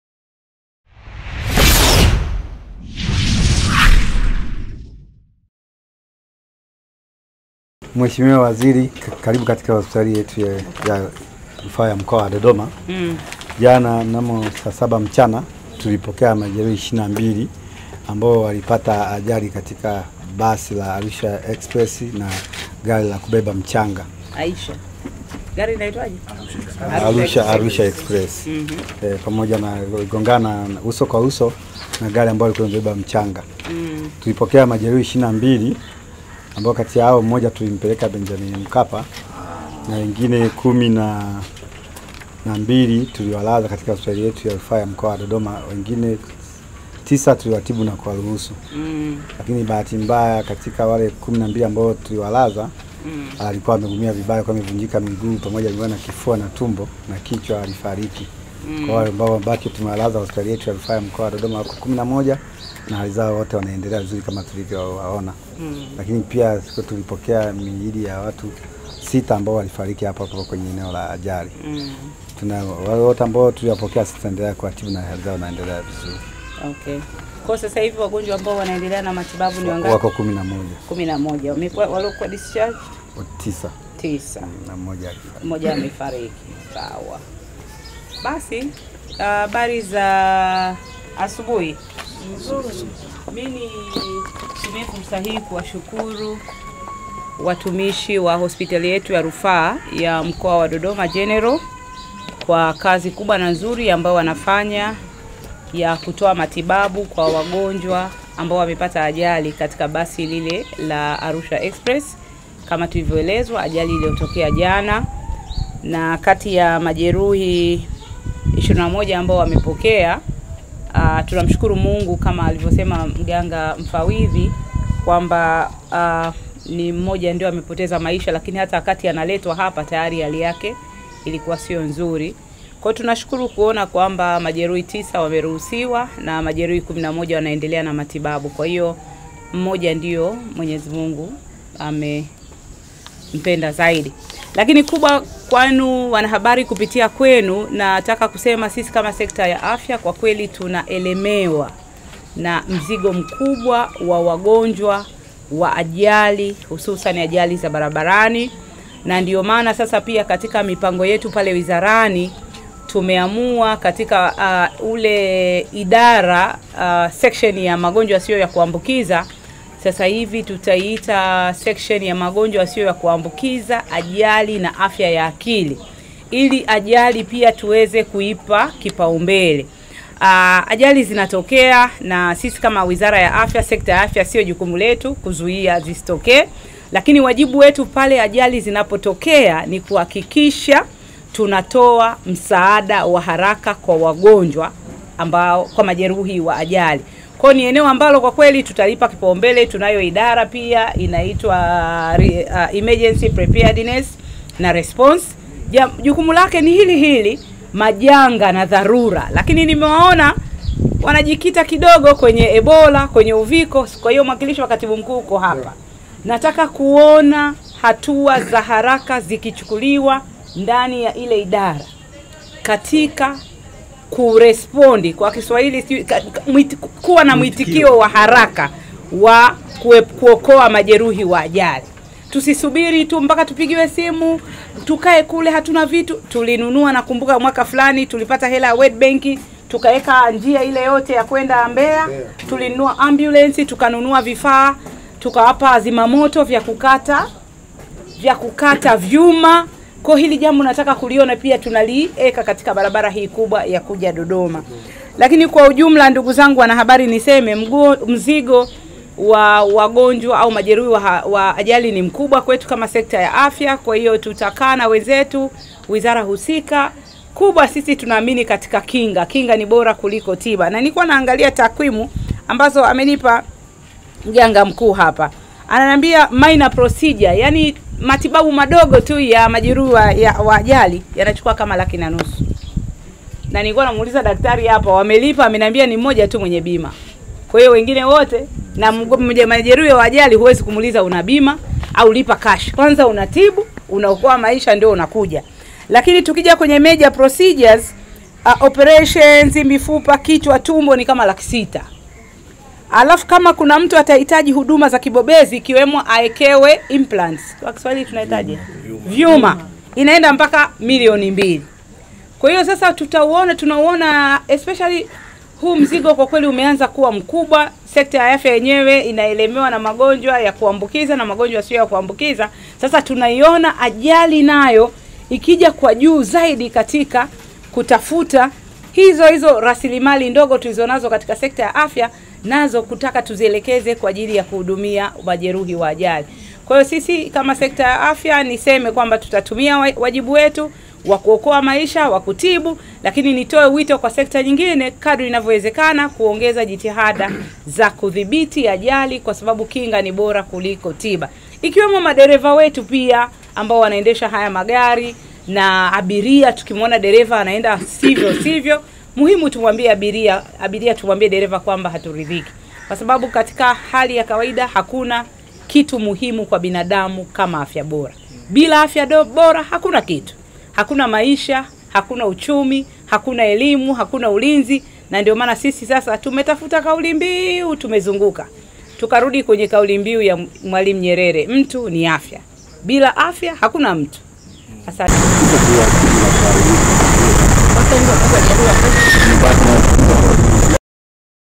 WAZIRI UMMY AKIRI WIZARA ya AFYA KUELEMEWA na WAGONJWA wa AJALI, ATOA MAAGIZO YA HARAKA...
Waziri wa Afya Ummy Mwalimu Amewatembelea majeruhi wa Ajali iliyohusisha Basi la Arusha Experes linalofanya safari zake Arusha Mbeya iliyotokea November 13, Katika kijiji cha zanka mkoa wa Dodoma kwa kugongana uso kwa uso na roli la mchanga ambapo watu sita walipoteza maisha papo hago na majeruhi 28

Waziri ummy amesema kwa sasa Wizara yake inalichukua tatizo la ajali kama jambo la dharula kwa kuwa hosptal nyingi zimeelewa na wangonjwa wanaotokana na ajali za barabarani.